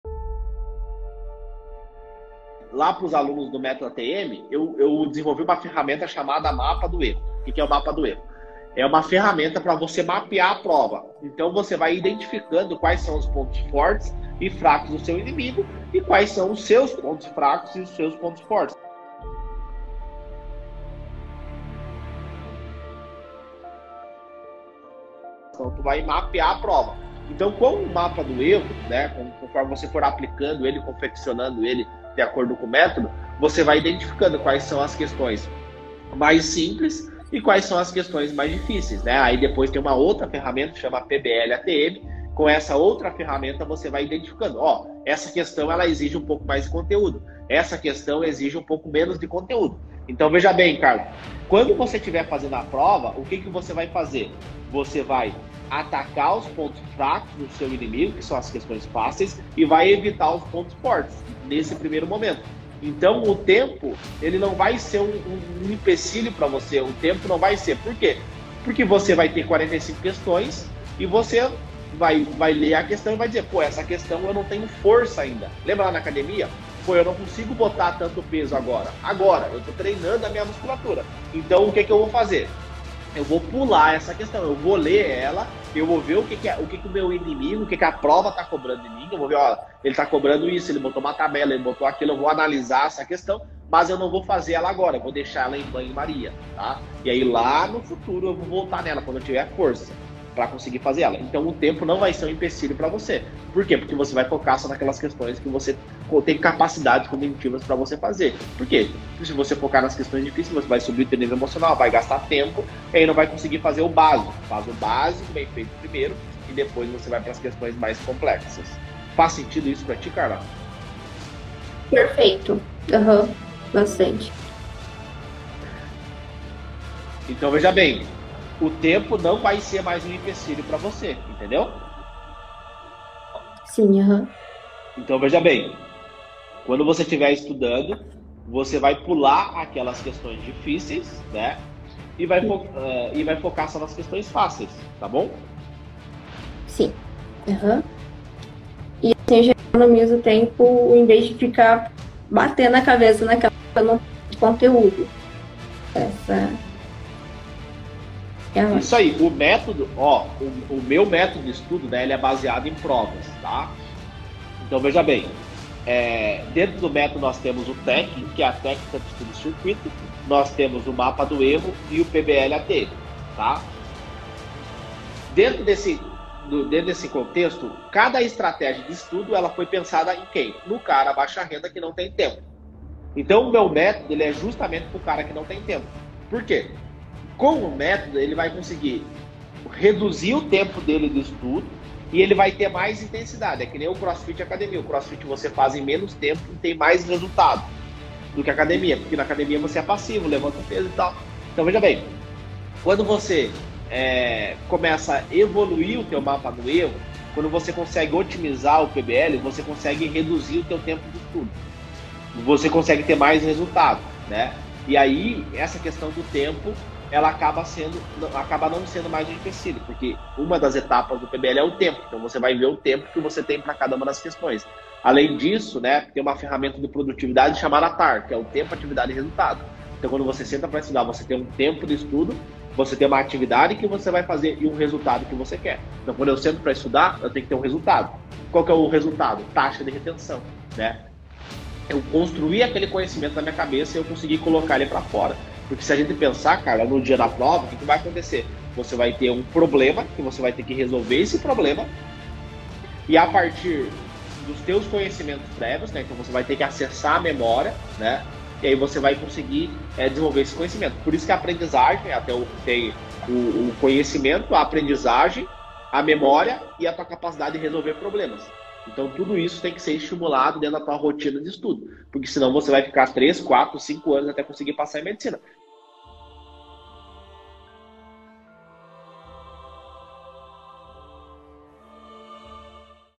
Inacreditável como essa ferramenta me fez passar em MEDICINA DE GRAÇA
Nesse vídeo, eu falo sobre como passei em Medicina estudando sozinho em casa no enem com essa ferramenta de estudo para passar em Medicina em 1 ano. Inacreditável como essa  ferramenta me fez passar em Medicina de graça.

Abraço
Leonardo Vicari